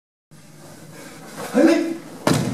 Yoko Shiho Nagare - Jujutsu
Jujutsu: The art of taking the opponent’s center. Jujutsu focuses on joint locks, throws, takedowns, chokes and strikes.

Come train with us in Liberty Hill, Texas!

Visit our other channel to check out our content on armed self defense at @urbandefenseacademy_texas